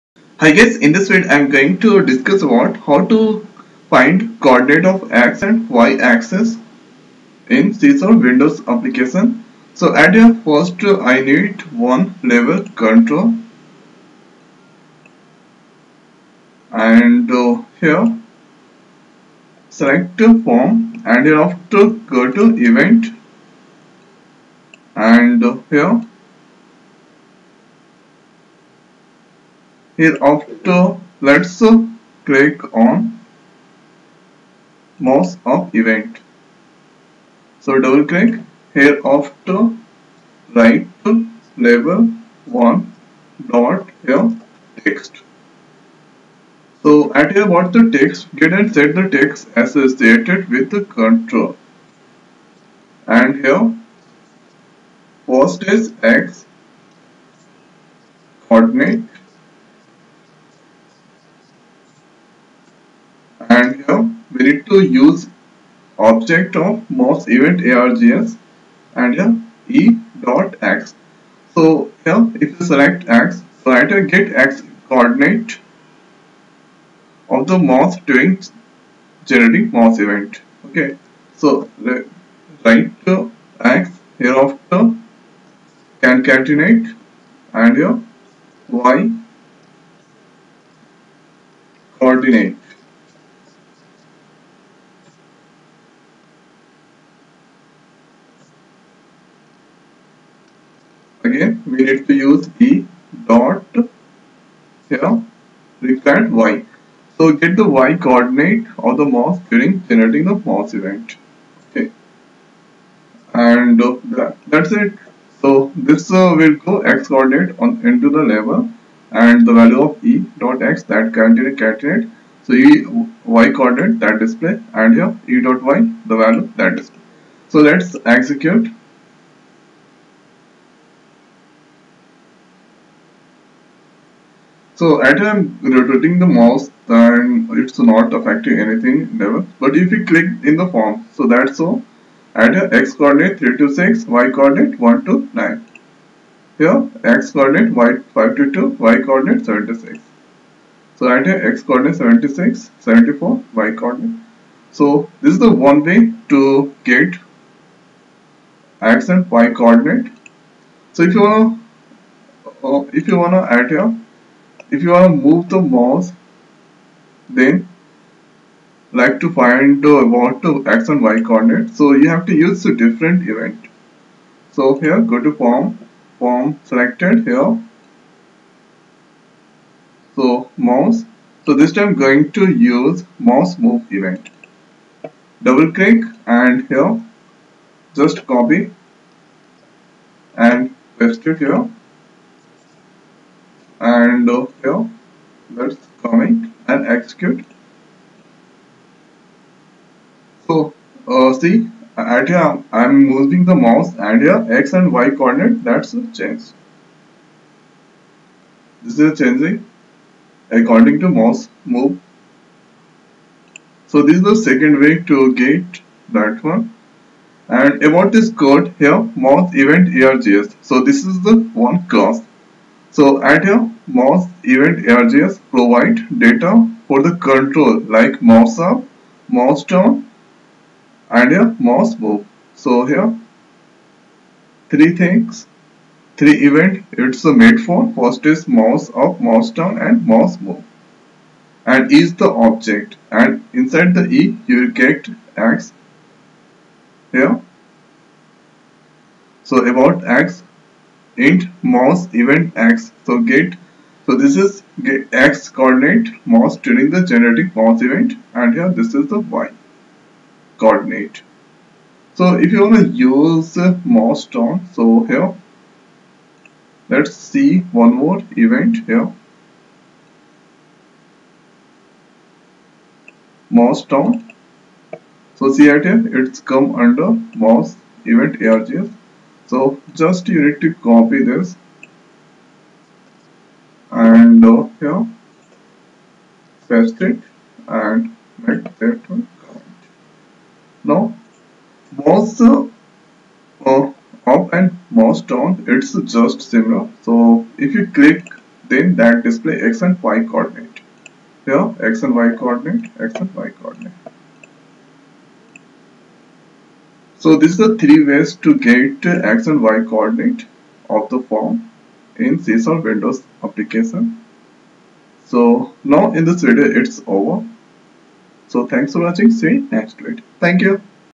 C# How to display X and Y coordinates
Display X & Y Mouse Coordinates Of Your Form in C#
C# Printing Using X & Y Coordinates